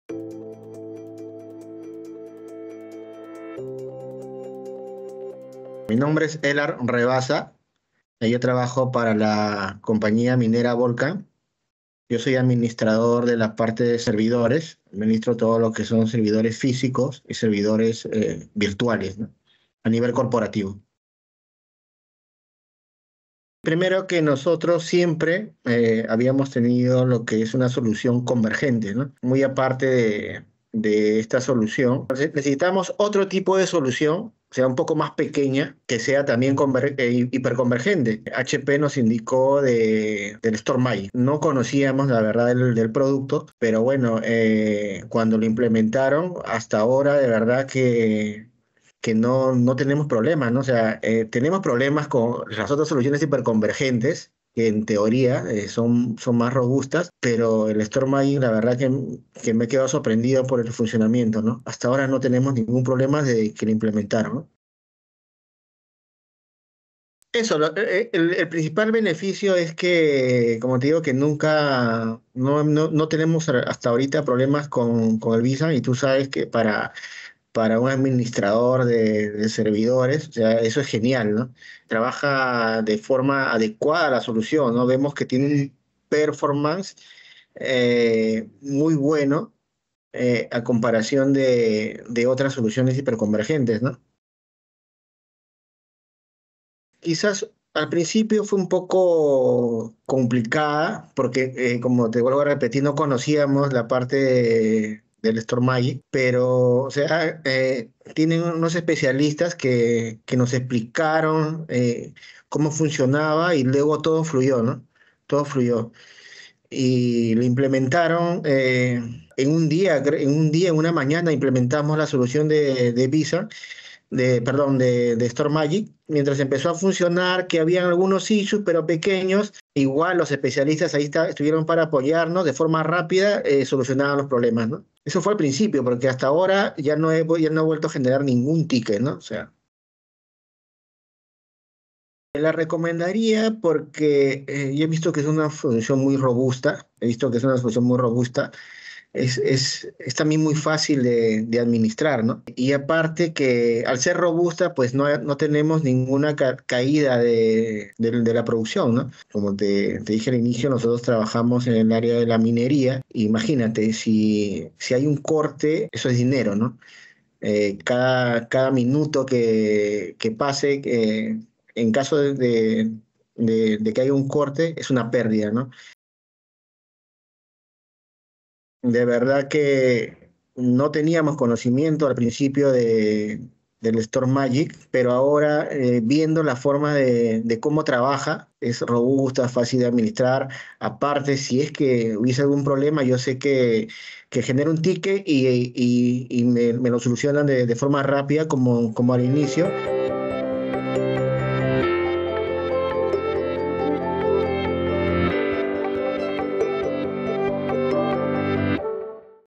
Volcan Customer Testimonial (Elard Rebasa)
Hear from Elard Rebasa, Server Administrator, as he shares his experience with StorMagic SvSAN - how it addressed the problems his organization was facing, and why he would recommend StorMagic to others.

00:00 Introduction
00:04 Q1 - Name and role
00:21 Q2 - Challenges
01:05 Q3 - Benefits
01:34 Q4 - Installation
02:20 Q5 - Recommendation
02:56 Q6 - Additional details
03:20 Outro